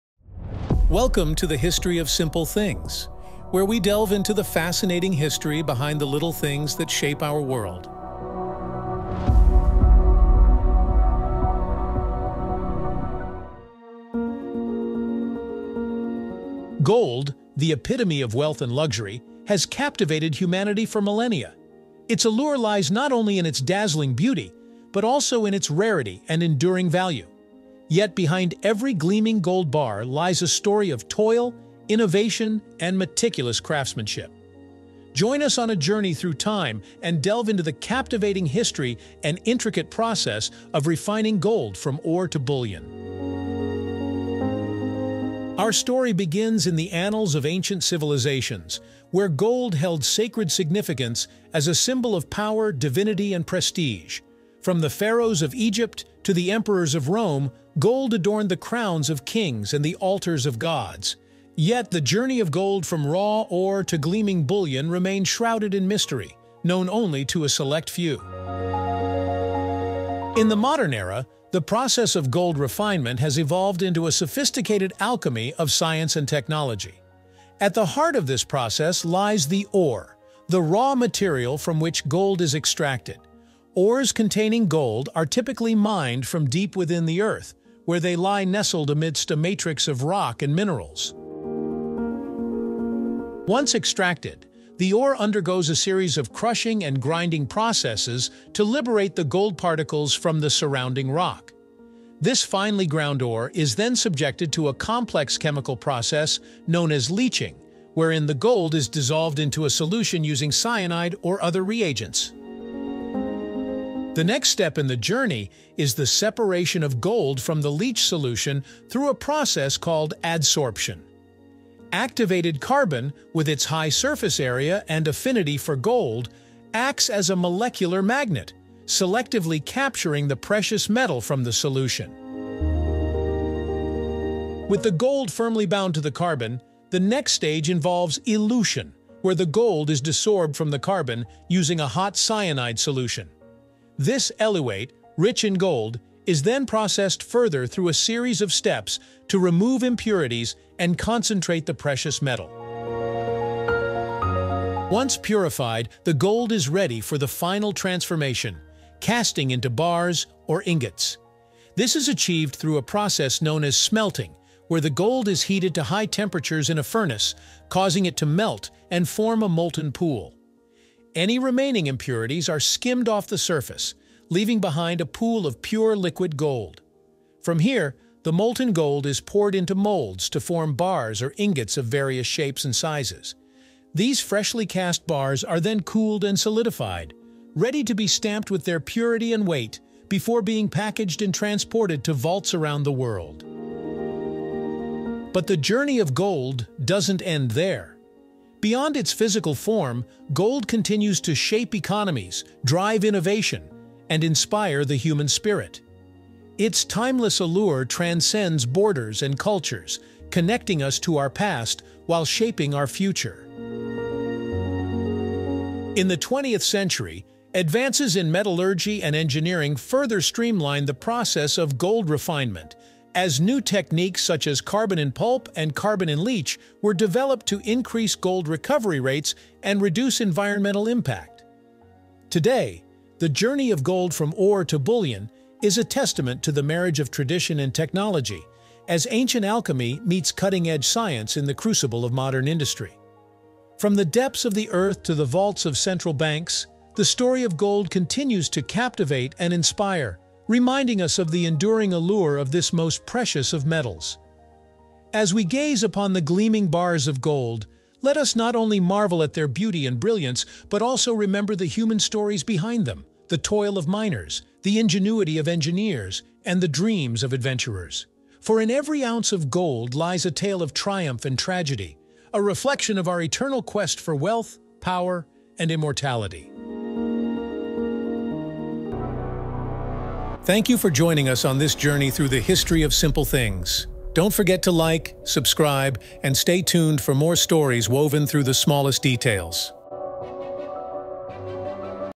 From Ore to Gold Bars: The Fascinating Journey of Gold Refinement
Step into the enchanting world of gold refinement with our latest video. Join us on an extraordinary voyage through time and across continents as we uncover the intricate process of transforming raw ore into gleaming Gold bars.

In this captivating exploration, we delve into the art of gold refinement – a process steeped in tradition and innovation. From the depths of the earth to the vaults of nations, we unravel the secrets of extracting, purifying, and casting gold into its coveted form.

As we bid farewell to the gleaming bars that symbolize wealth and power across the ages, we invite you to join us on this mesmerizing odyssey of discovery.